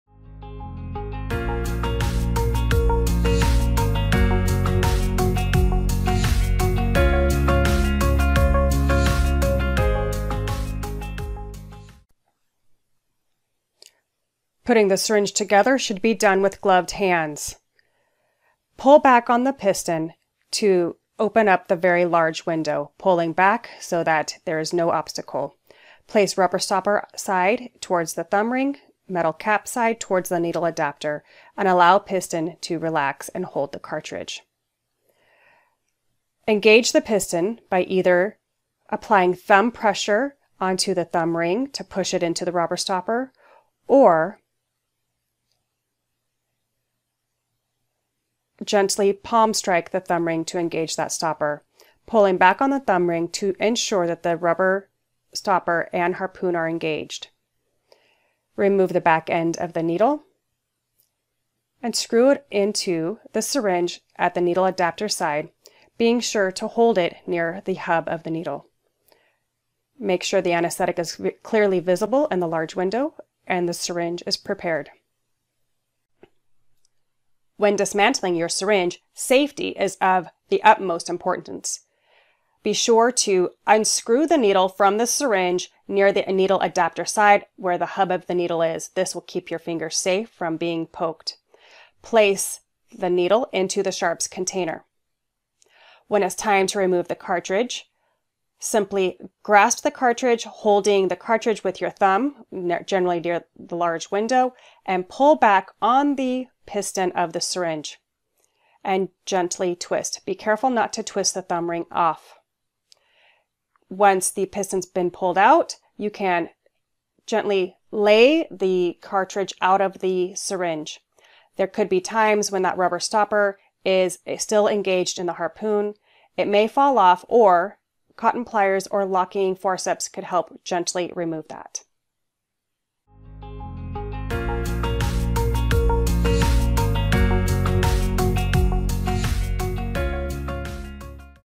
SYRINGE ASSEMBLY
A basic overview of preparing a dental syringe for use and safely dismantling after use. Proper clinical safety and infection control procedures must be followed at all times.